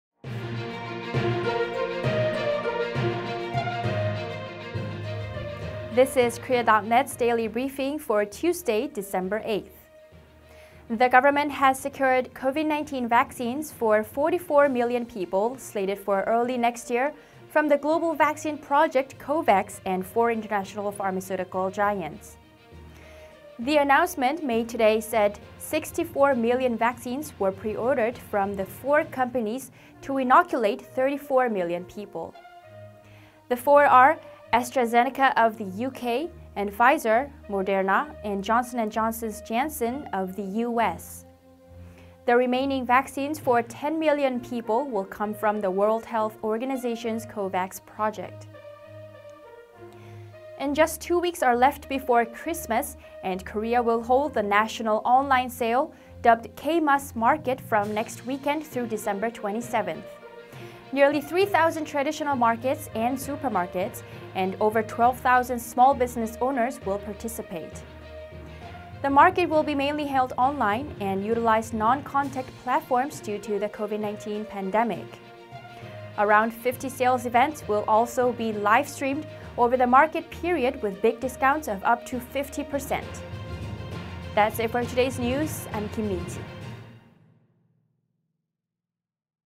Gov't secures vaccines for 44M people next year - 한국, 코로나 백신 4천4백만 명분 확보
✔️Gov't secures vaccines for 44M people next year
The government has secured COVID-19 vaccines for 44 million people slated for early next year from the global vaccine project COVAX and four international pharmaceutical giants.
The announcement made today said 64 million vaccines were preordered from the four companies to inoculate 34 million people.
The four are AstraZeneca of the U.K. and Pfizer, Moderna and Johnson & Johnson's Janssen of the U.S.
The remaining vaccines for ten million people will come from the World Health Organization's COVAX project.

✔️정부, 코로나 백신 4천4백만명분 확보···내년 2~3월 단계적 도입
정부가 내년 초 백신공급기구 코백스 프로젝트와 해외 제약사를 통해 도입될 백신 4천4백만 명분을 확보했다고 전했습니다.
정부는 오늘 6천4백만 명분의 백신이 3천4백만명을 접종하기 위해 4개 백신 기업을 통해 선구매한다고 밝혔습니다.
네개는 영국 아스트라제네카, 미국의 화이자, 모더나, 얀센입니다.
천만명을 위한 백신은 세계보건기구의 코백스 프로젝트로부터 제공될 예정입니다.

✔️Online Xmas sale to start next weekend
Just two weeks are left before Christmas, and Korea will hold the national online sale dubbed "K-mas Market" from next weekend through Dec. 27.
Nearly 3,000 traditional markets and supermarkets and over 12,000 small business owners will participate in this event.
The market will be mainly held online and utilize non-contact platforms due to the COVID-19 pandemic.
Around 50 sales events will also be livestreamed over the market period with big discounts of up to 50%.

✔️비대면 크리스마스 마켓 다음 주말부터 열린다
크리스마스를 2주 남겨두고, 다음 주말부터 12월 27일까지 온라인 세일 'K-mas 마켓'을 엽니다.
약 3천개의 전통시장과 마켓, 1만2천의 소규모 자영업자들이 마켓에 참여할 예정입니다.
마켓은 코로나19로 인해 온라인과 비대면으로 이뤄집니다.